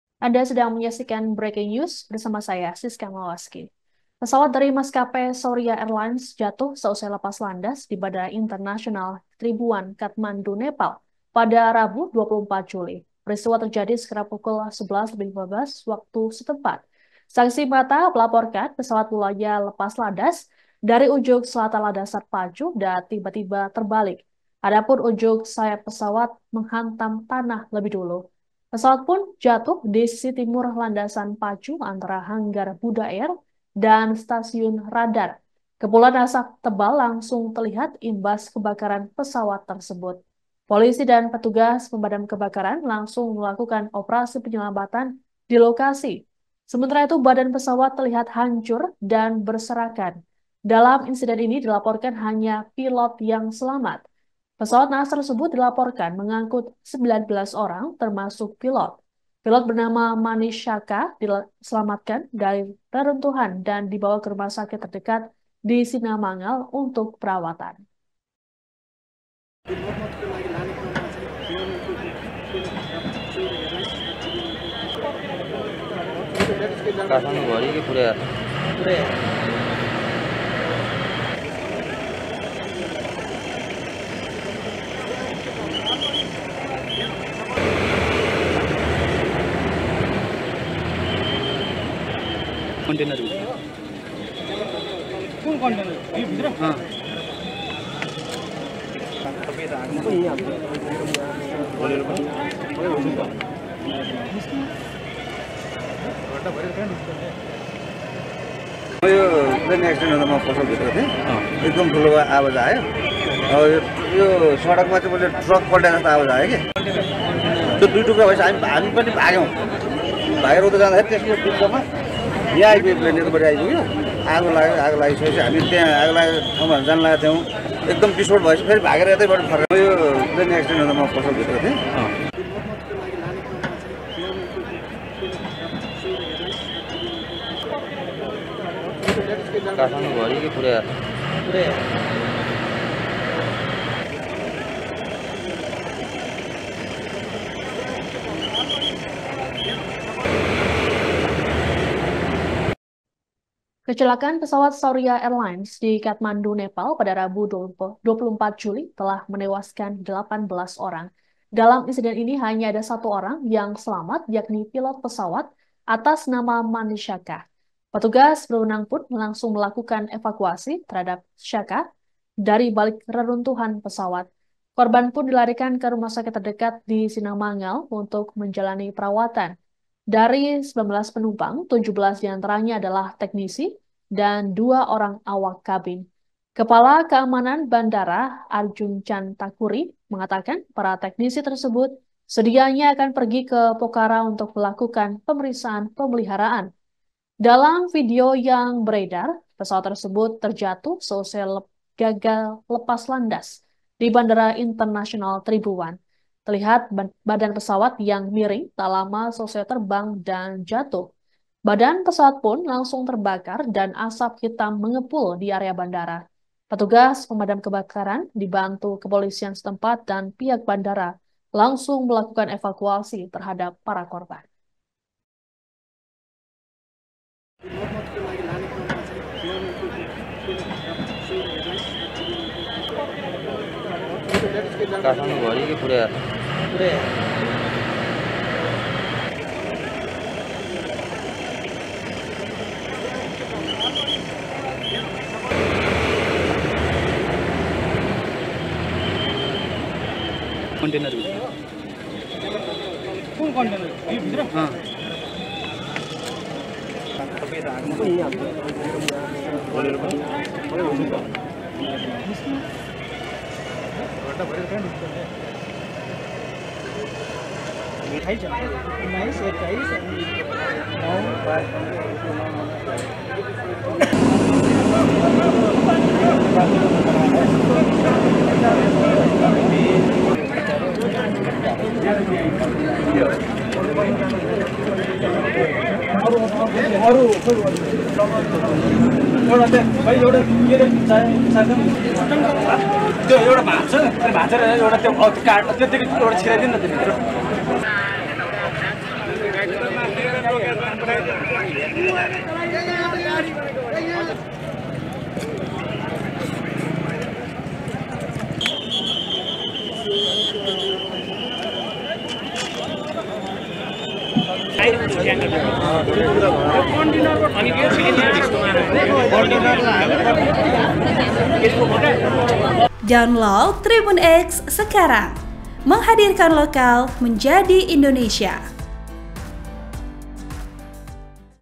Detik-detik Pesawat Saurya Airlines Jatuh Seusai Lepas Landas di Nepal, Hanya Pilot yang Selamat
TRIBUN-VIDEO.COM - Pesawat dari maskapai Saurya Airlines jatuh seusai lepas landas di Bandara Internasional Tribhuvan, Kathmandu, Nepal pada Rabu (24/7/2024).

Peristiwa terjadi sekira pukul 11.15 waktu setempat.

Saksi mata melaporkan, pesawat mulanya lepas landas dari ujung selatan landasan pacu, tiba-tiba terbalik.

Adapun ujung sayap pesawat menghantam tanah lebih dulu.

Pesawat pun jatuh di sisi timur landasan pacu antara hanggar Buddha air dan stasiun radar.

Kepulan asap tebal langsung terlihat imbas kebakaran pesawat tersebut.

Polisi dan petugas pemadam kebakaran langsung melakukan operasi penyelamatan di lokasi.

Sementara itu, badan pesawat terlihat hancur dan berserakan di lokasi.

Dalam insiden ini, dilaporkan hanya pilot yang selamat.

Pesawat nahas tersebut dilaporkan mengangkut 19 orang termasuk pilot.

Pilot bernama Manish Shakya diselamatkan dari reruntuhan dan dibawa ke rumah sakit terdekat di Sinamangal untuk perawatan.

(TribunVideo.com)

Program: Viral News
Host: Fransisca Krisdianutami Mawaski
Editor Video: Okwida Kris Imawan Indra Cahaya
Uploader: Aprilia Saraswati